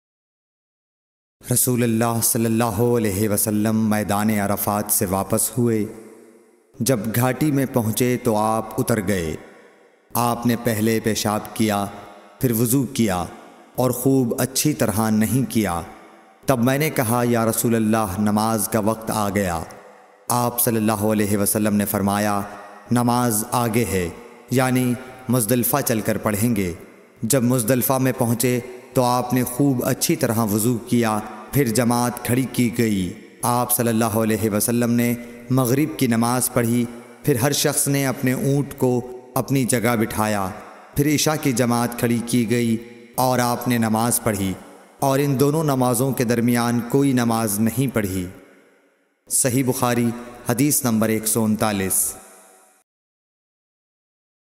Rasool Allah ﷺ ka Muzdalfah mein wudu or Namaz Ka Triqa ?
❶ Narrated Usama bin Zaid: Allah's Apostle proceeded from `Arafat till when he reached the mountain pass, he dismounted, urinated and then performed ablution but not a perfect one. I said to him, (  Is it the time for) the prayer, O Allah's Apostle?   He said,   The (place of) prayer is ahead of you.   He rode till when he reached Al-Muzdalifa, he dismounted and performed ablution and a perfect one, The (call for) Iqama was pronounced and he led the Maghrib prayer. Then everybody made his camel kneel down at its place. Then the Iqama was pronounced for the `Isha' prayer which the Prophet led and no prayer was offered in between the two . prayers (`Isha' and Maghrib).

THE BOOK OF WUDU (ABLUTION).
Status: صحیح